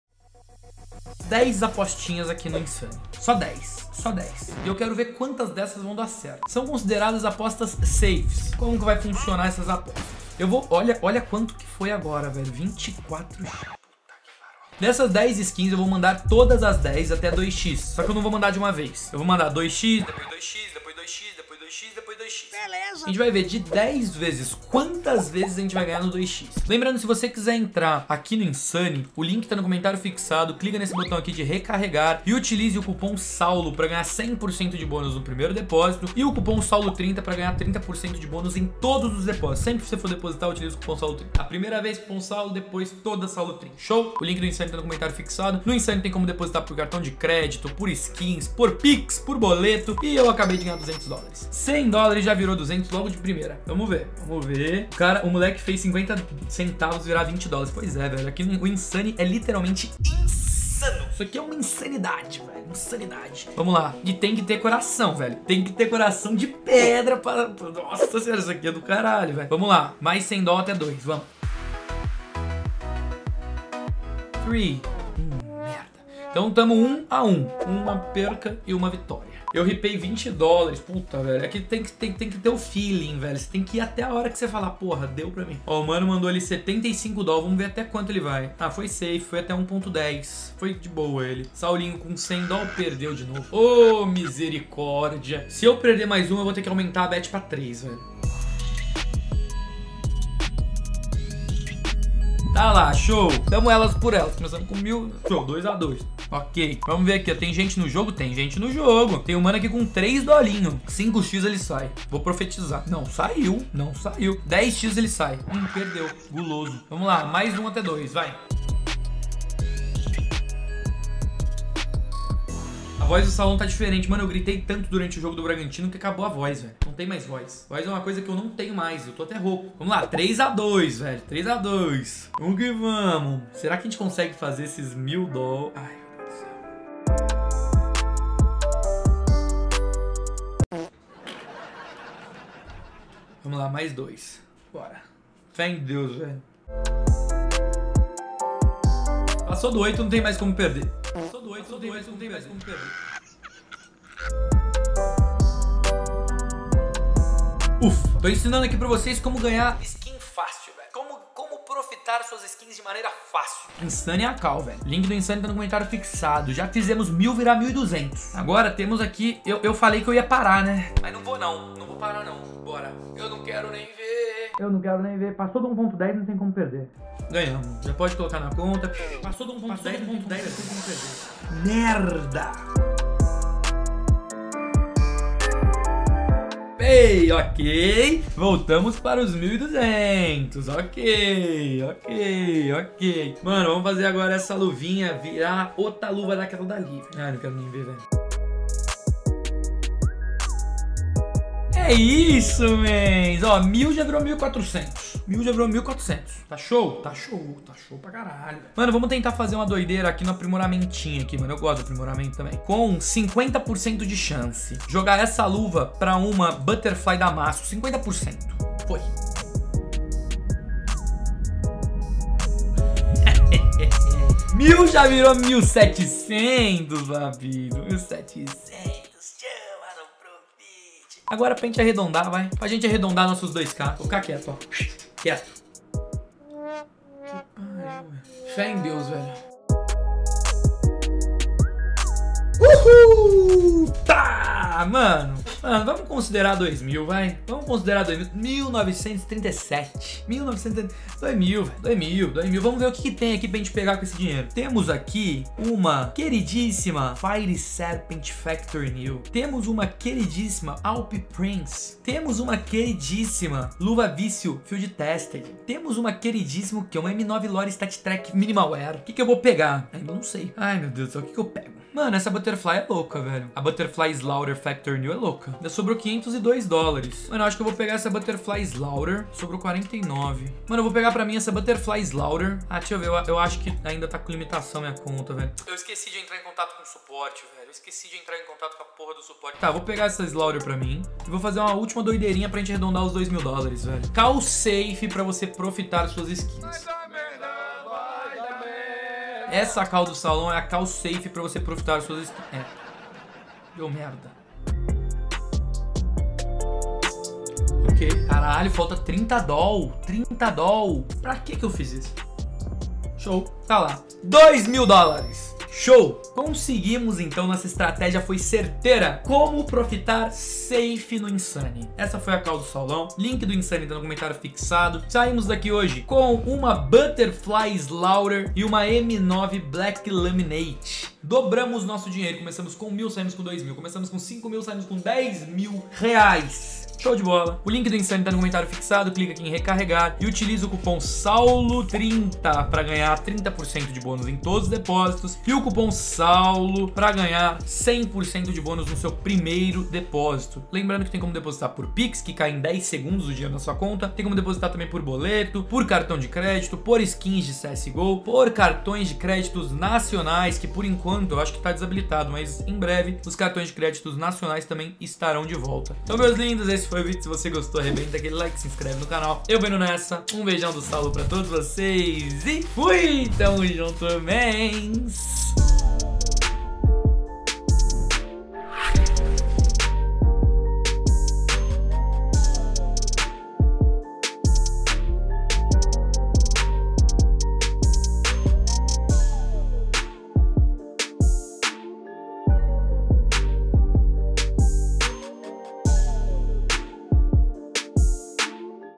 Como ganhar SKINS DE CSGO em 2021... Método SAFE de PROFIT
Ensinei a ganhar skins de CSGO